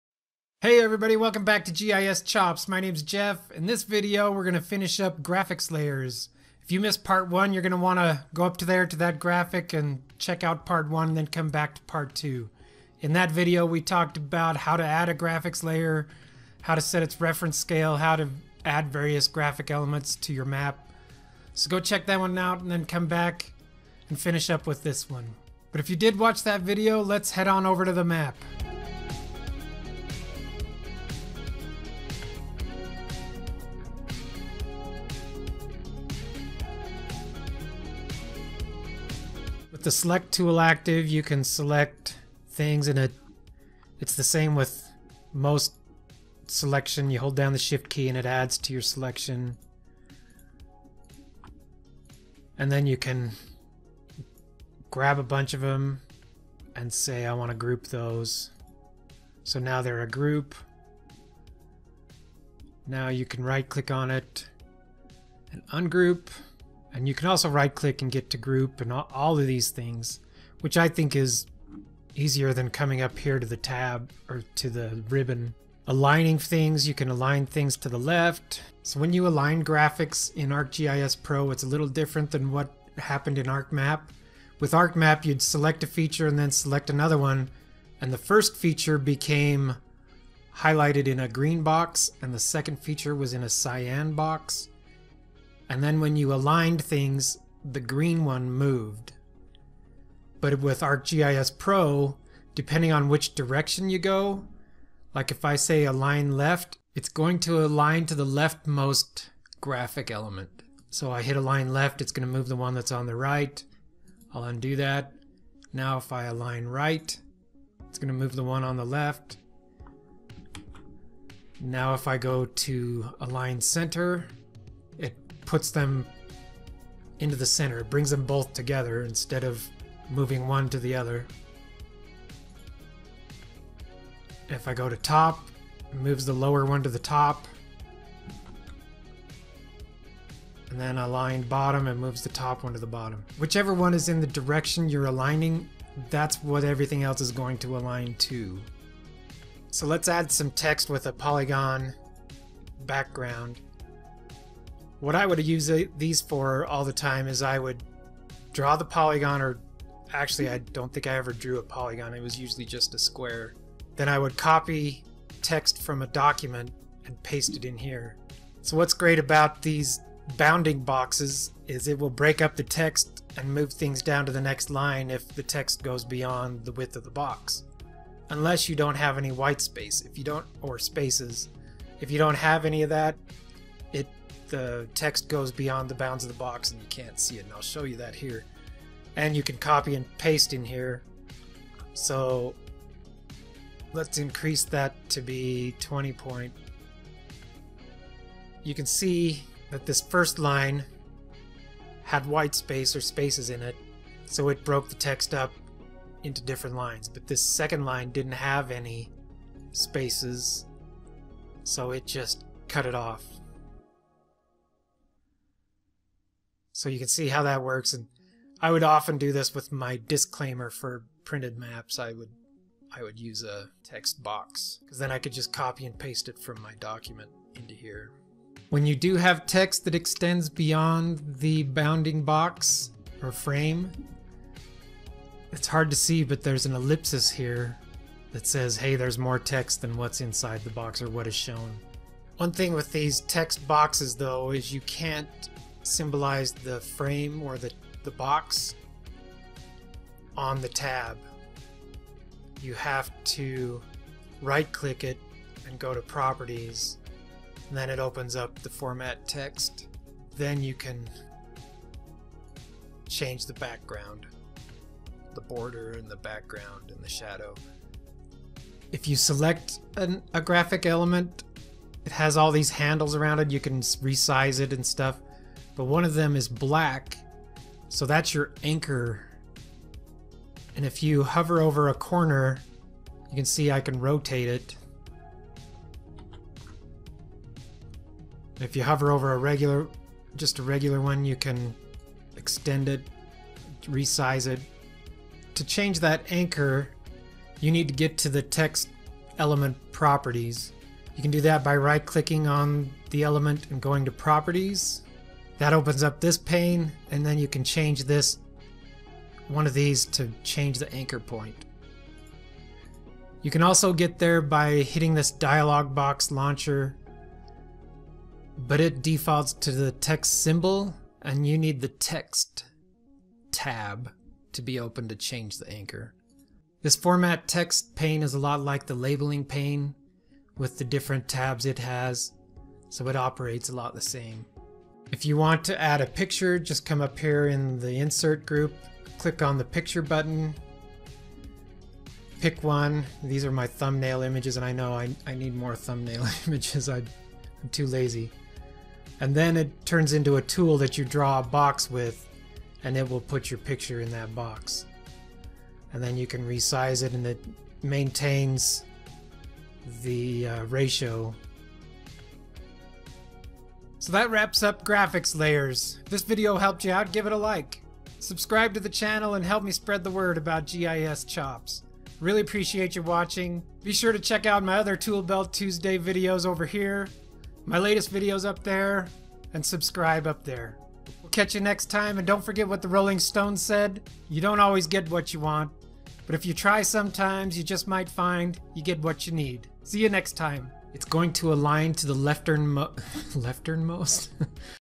GIS Skills: ArcGIS Pro Introduces Graphics Layers Part 2
Get serious GIS Skills by learning how to add and manipulate graphics layers. ArcGIS Pro now lets you add graphics layers to your map. With graphics layers you can add graphic elements to your maps, which is a quick way to draw attention to a specific area or make notes or quick annotations without having to create a feature class. You can add polygons, lines, points, text and pictures.